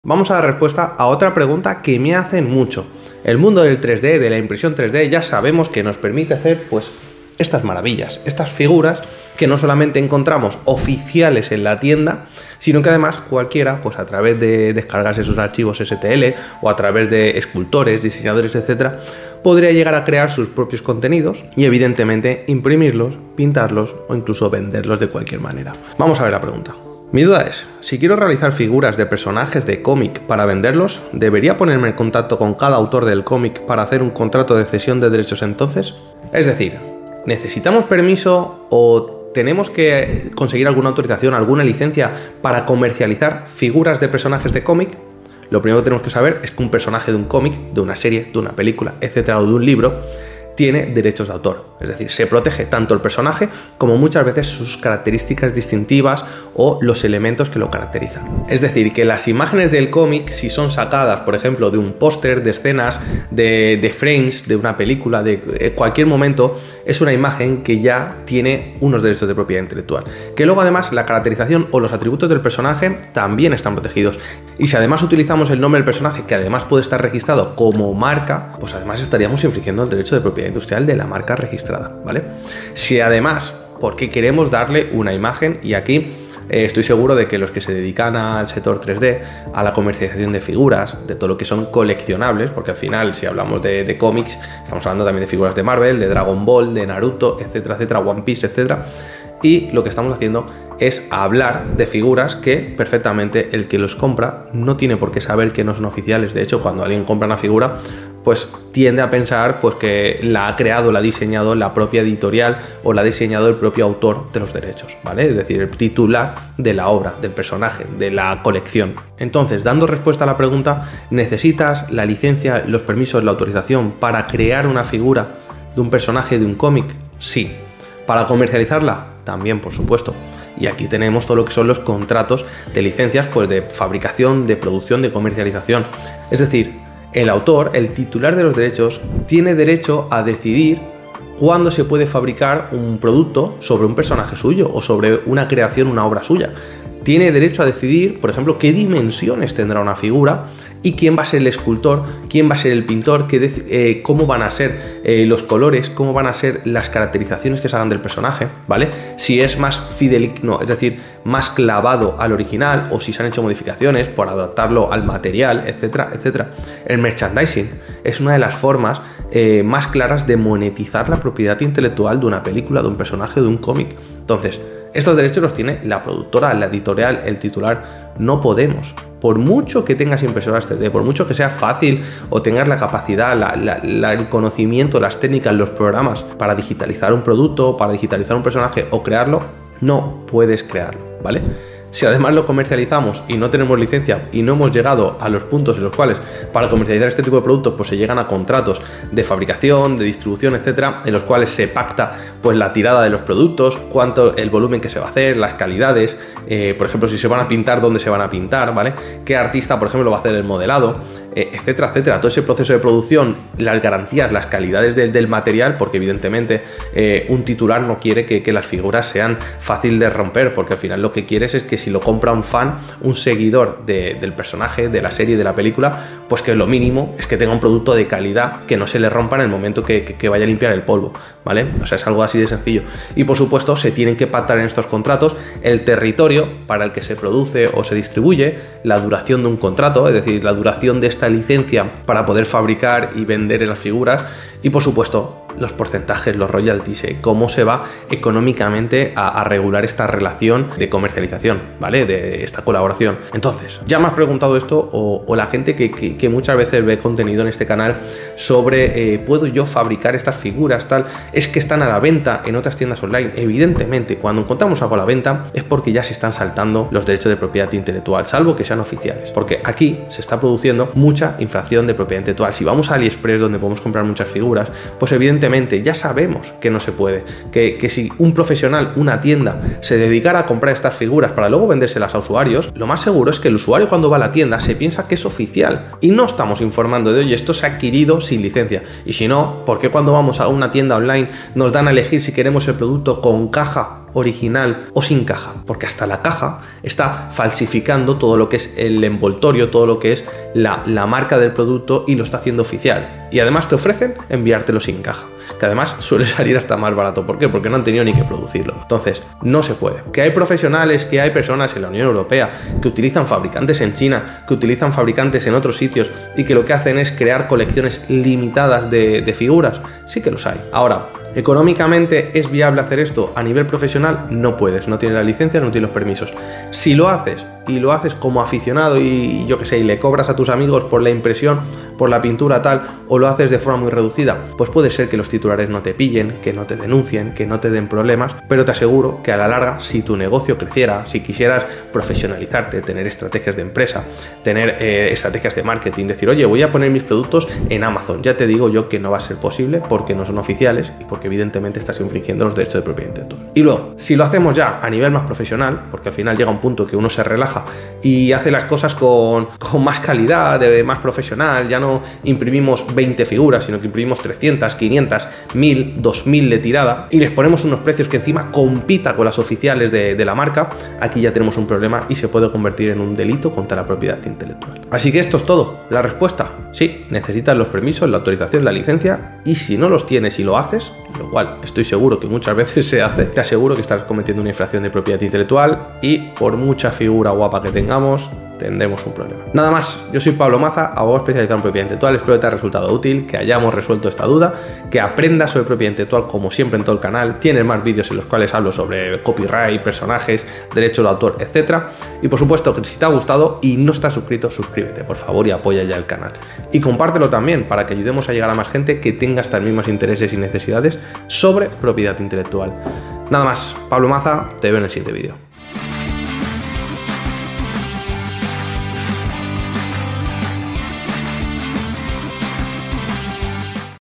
Propiedad Intelectual en FIGURAS y 3D
Cuidado con las figuras de personajes en 3D y Juguetes. Tienen derechos de propiedad intelectual y están protegidos.

Desde los derechos de autor hasta las marcas registradas, las figuras 3D tienen que respetar los derechos de otros 🚀

Explico si puedes crear figuras de Dragon Ball, Naruto, One Piece y otros Animes, Mangas, Series, Películas y Juegos.